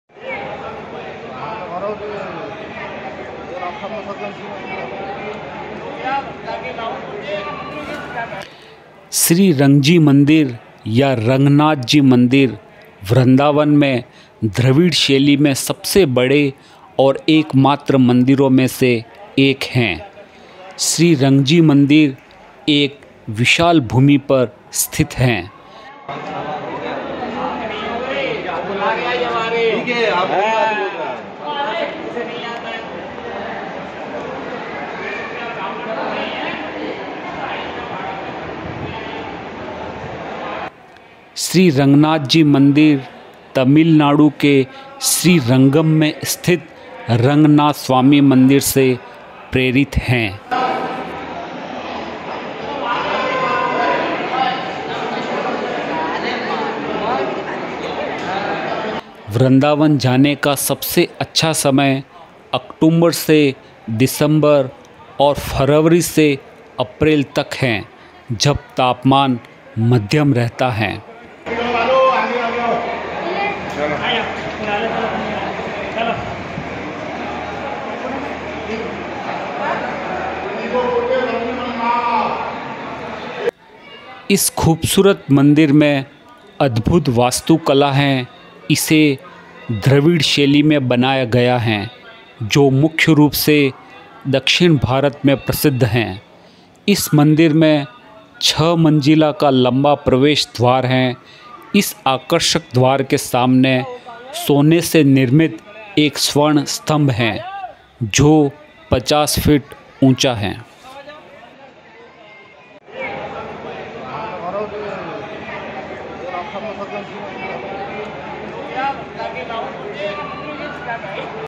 रंगजी //रंगनाथ जी मंदिर, वृन्दावन //Rangnath Temple, Vrindawan
द्रविड़ शैली, सोने का खम्भा, छ मंजिला प्रवेश द्वार,